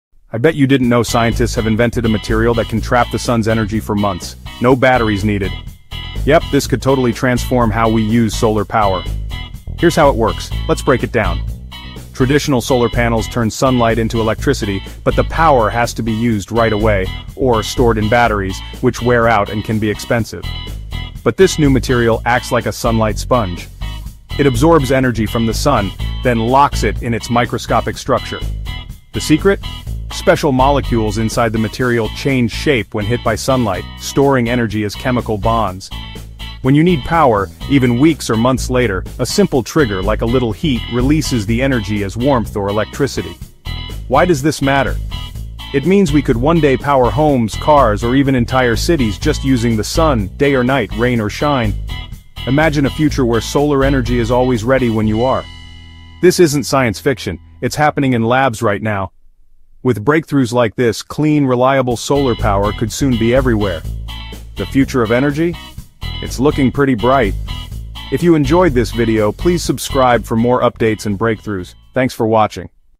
Revolutionary Solar Material: Energy Storage Breakthrough!
Hello Everyone..!!!
Discover the future of renewable energy in this captivating YouTube Short! 🌞 Dive into the world of groundbreaking materials that can store solar energy for months—without batteries! Through stunning visuals of sunlight bathing solar panels, microscopic footage of innovative materials, and scientists in action, we’ll unveil how this revolutionary technology works in simple terms.

Learn why storing solar energy matters for our everyday lives and the potential it holds for a sustainable future.

Join us on this incredible journey toward a greener tomorrow!